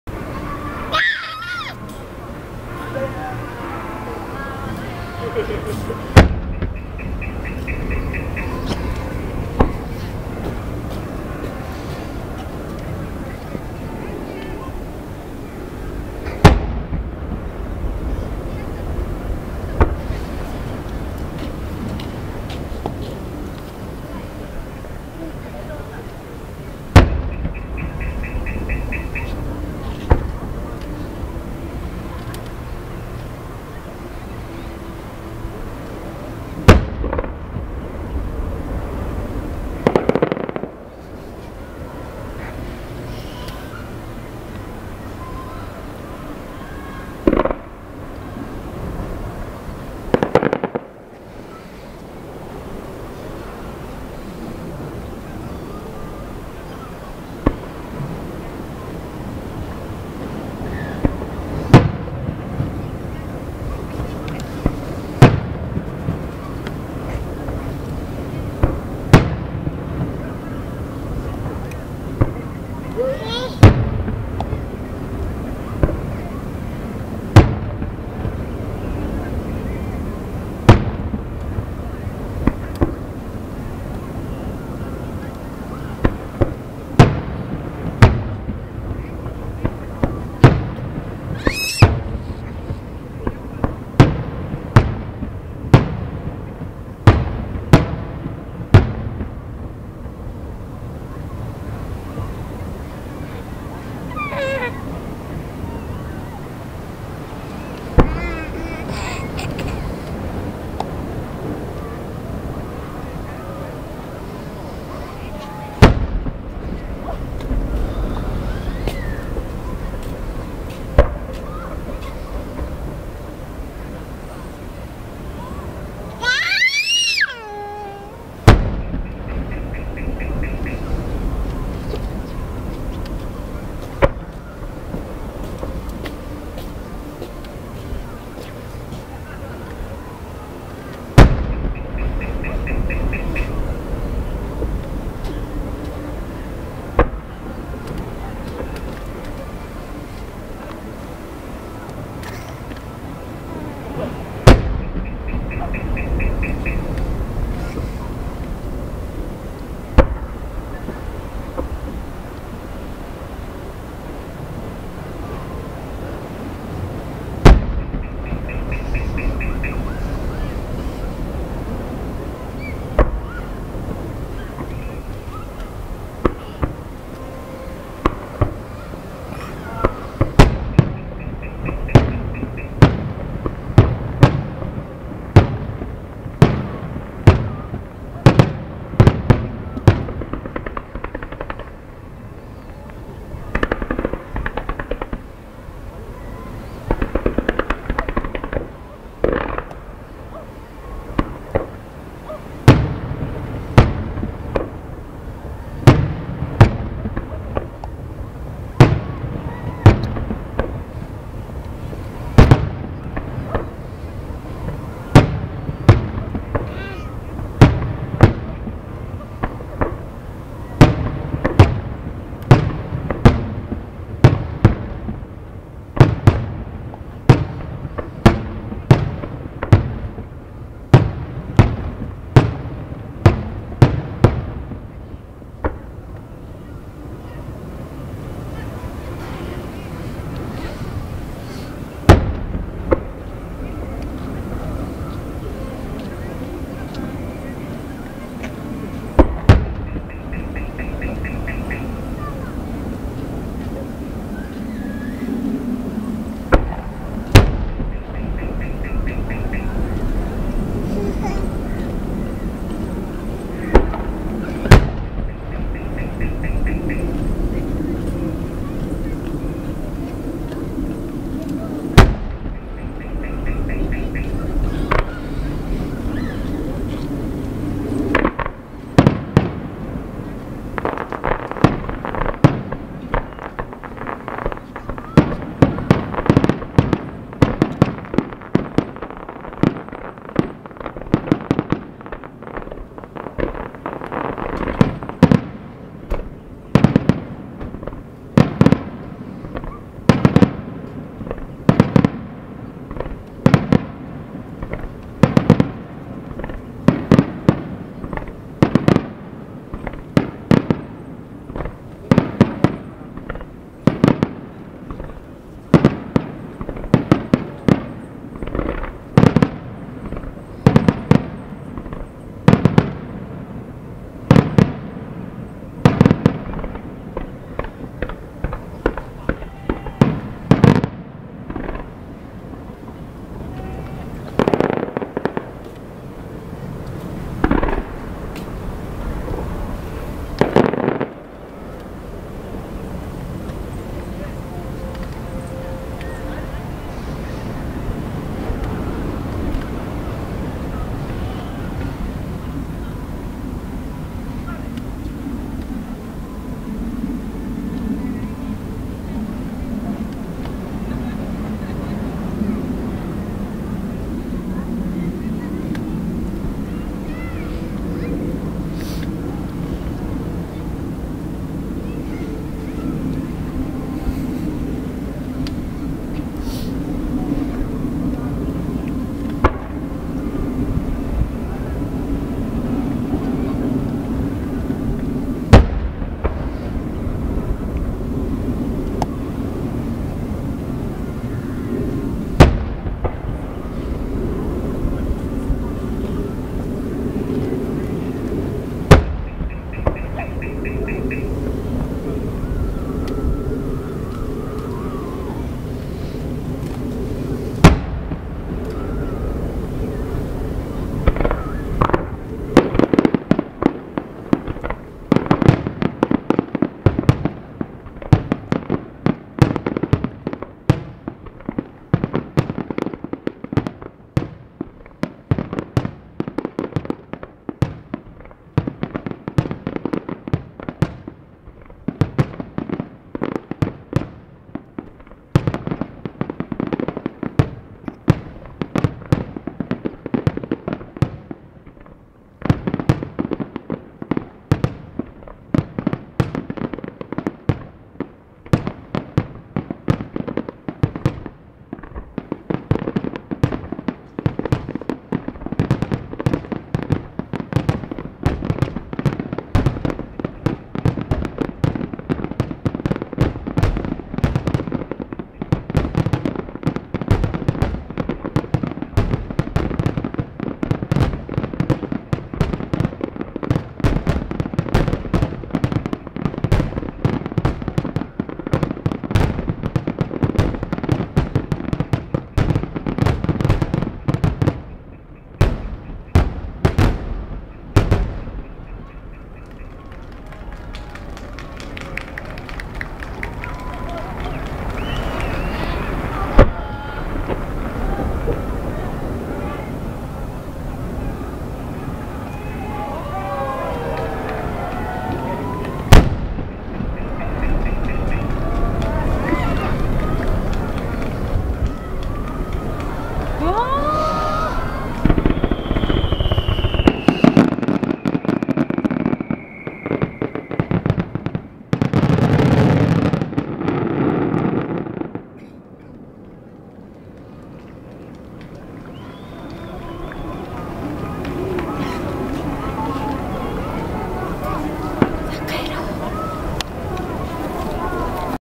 ふじさわ江ノ島花火大会フィナーレ 2014/10/18- Part.2 (Fireworks at Enoshima 2014)
湘南の江ノ島で、2014年10月18日（土）に開催された花火大会。約3,000発の花火が打ち上げられ、湘南海岸では唯一見られる2尺玉が有名。
A display of fireworks at Enoshima, about 3,000 were launched.